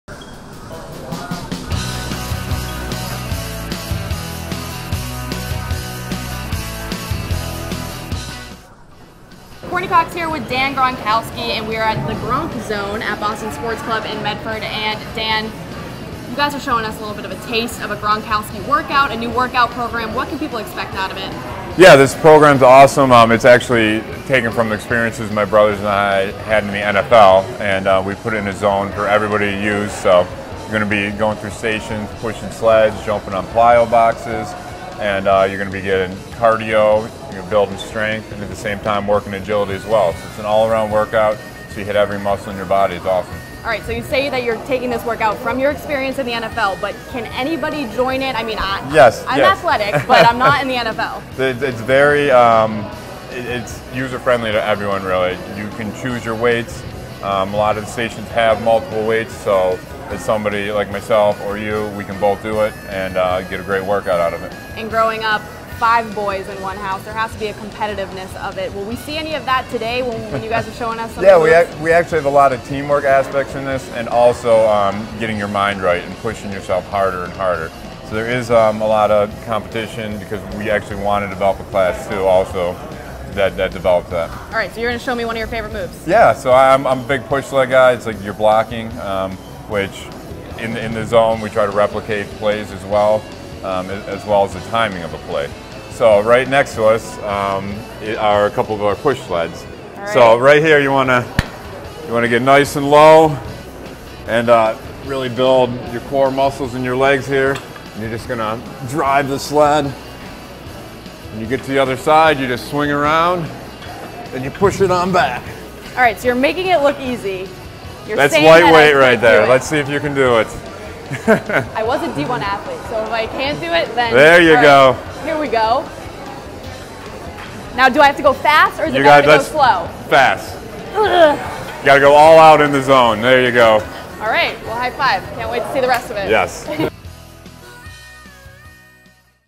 NESN.com Takes On The Gronk Fitness Program
New England Patriots tight end Rob Gronkowski and family are athletic ... to say the least. NESN.com's Courtney Cox went through an hour of the workout, and spoke with Dan Gronkowski to learn more about what goes into the NFL-inspired program.